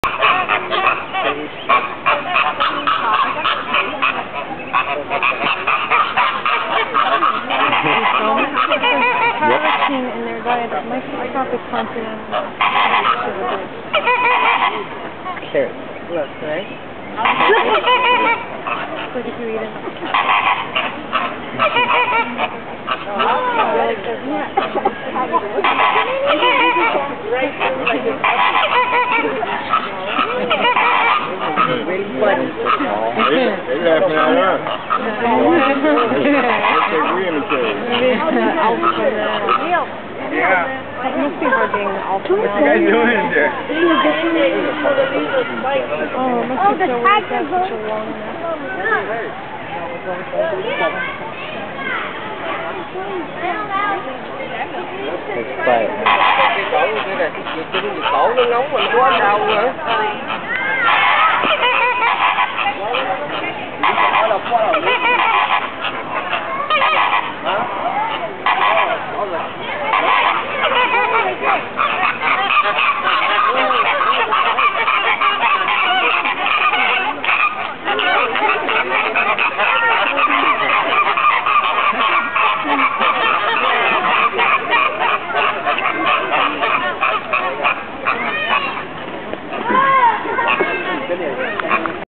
Pink Flamingos -- the real ones
A short clip of the very social flamingos at the Bronx Zoo.